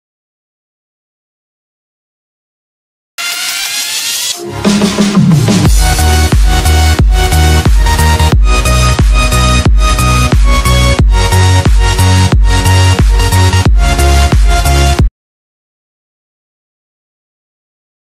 Best 2D intro without text
hello friends this is only one intro but I like it very much and I suggest you to put it as your intro. This is a 2D intro.